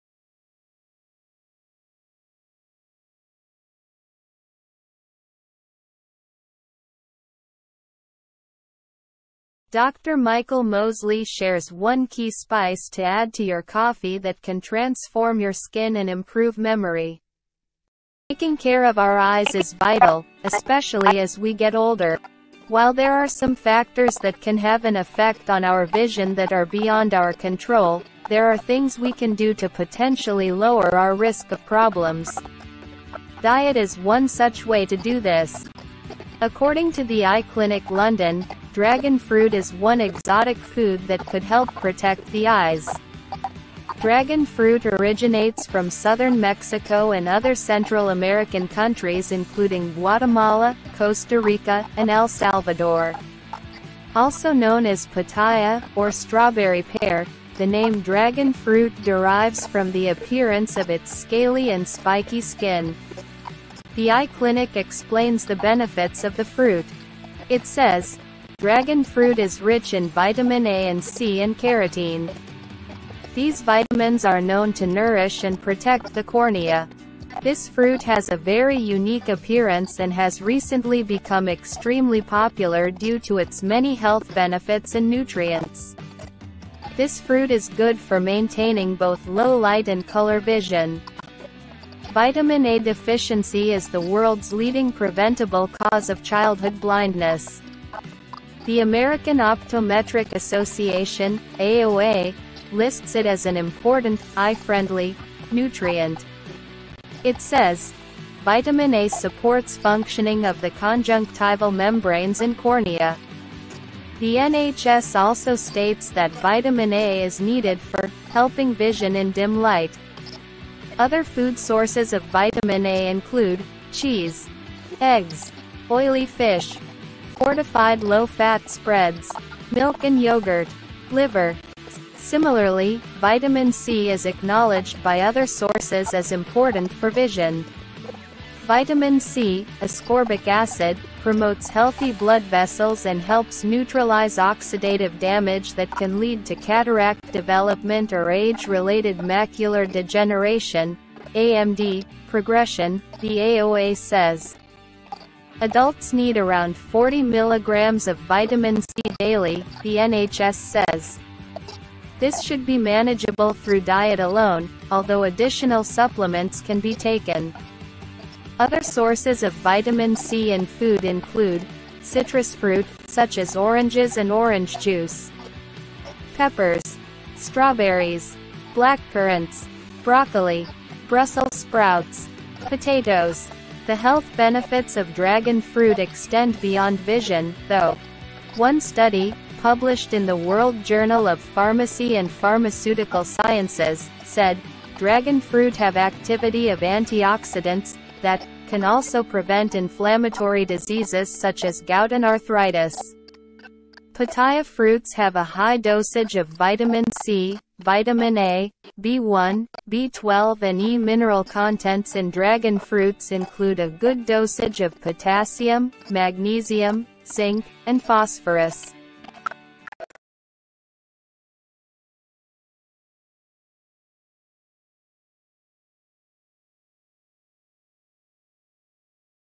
DR MICHAEL MOSLEY SHARES ONE KEY SPICE TO ADD TO YOUR COFFEE THAT CAN TRANSFORM YOUR SKIN AND IMPROV
TV Doctor and presenter Michael Mosley has suggested incorporating one everyday spice to your coffee which could have some surprising health benefits. He suggested adding turmeric; a popular spice that is commonly used in Indian and Middle Eastern cuisine.